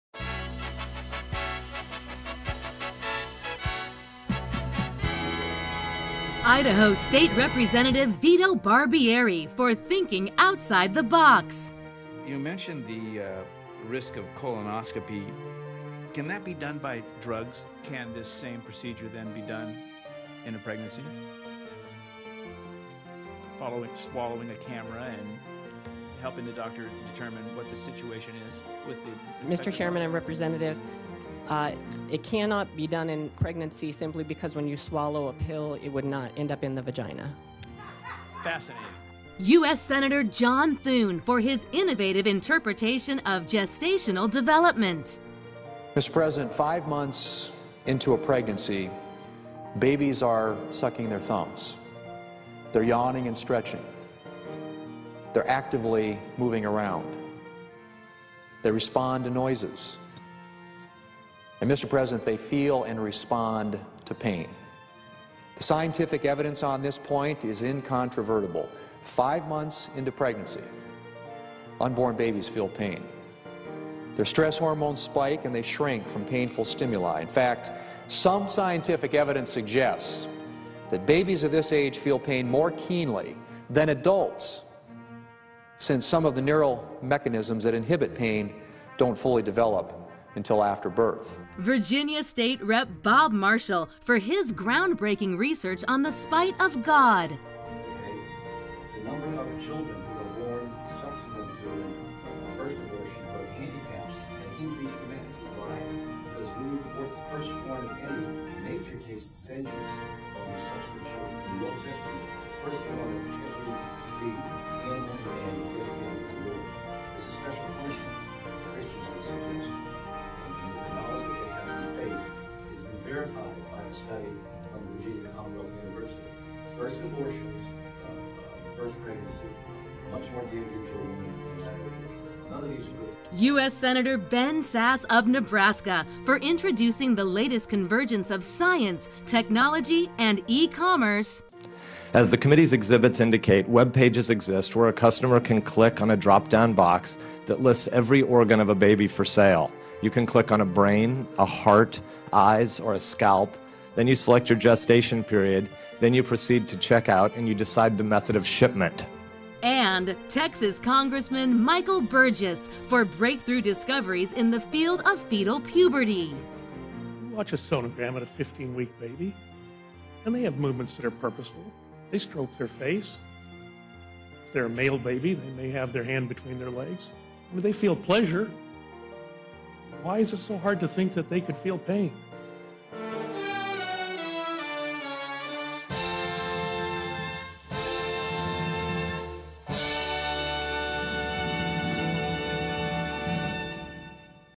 Best Original Science
The 2016 Golden Probe Awards nominees for "Best Original Science" include:
- Idaho State Representative, Vito Barbieri
- U.S. Senator, John Thune of South Dakota
- Virginia State Rep, Bob Marshall (WINNER)
- U.S. Senator, Ben Sasse of Nebraska
- Texas Congressman, Michael Burgess